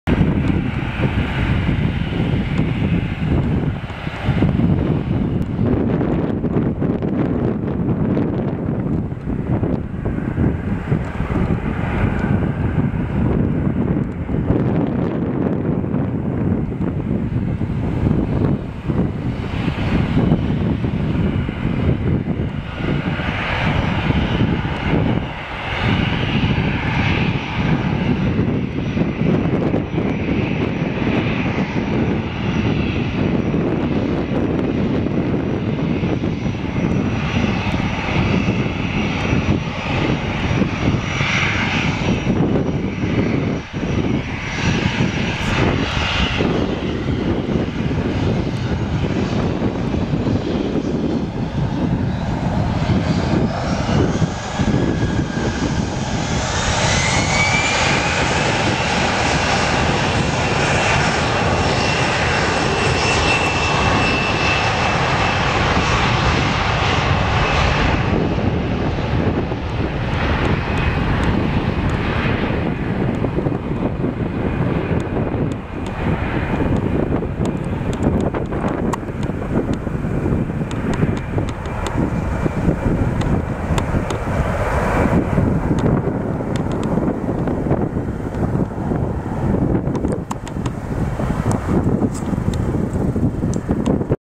Aerosweet take-off 36R UKBB
Взлет Аэросвита в Борисполе с 36 правой
Это моя первая съемка, штатив тугой и солнце било в глаза, поэтому качество для себя